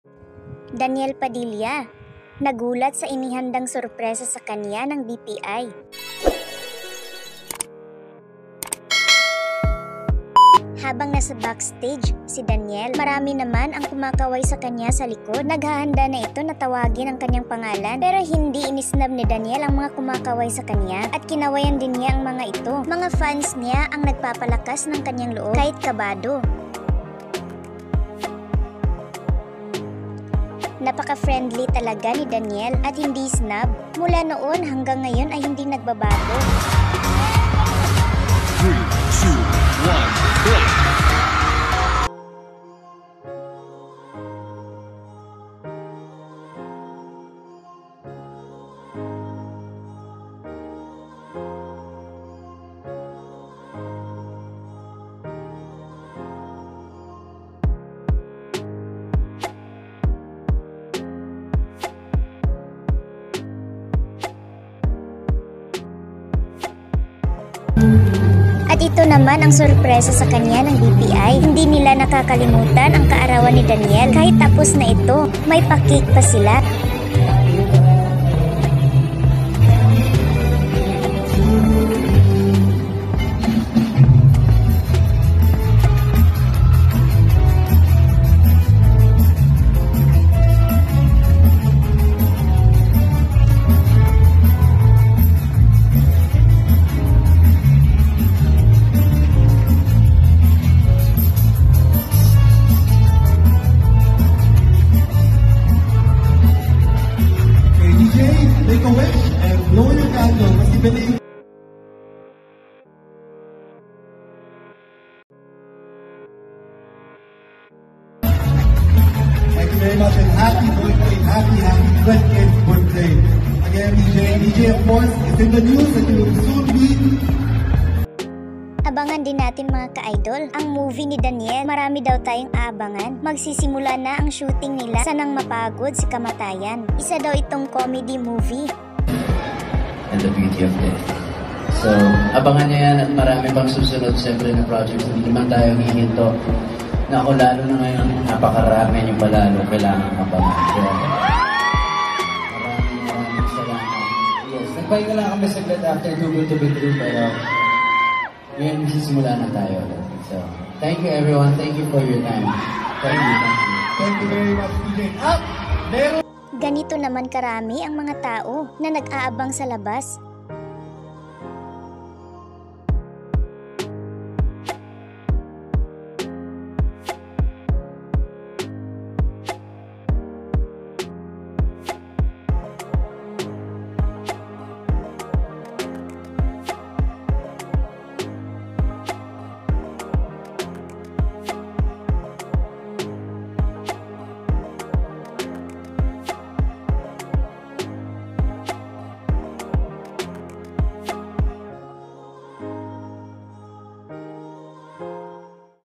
Daniel Padilla, nabigla sa sorpresa, sisimulan na daw ang taping! 😍😍😍
Daniel John Elago Ford born April 26, 1995, better known by his screen name Daniel Padilla , is a Filipino actor and recording artist.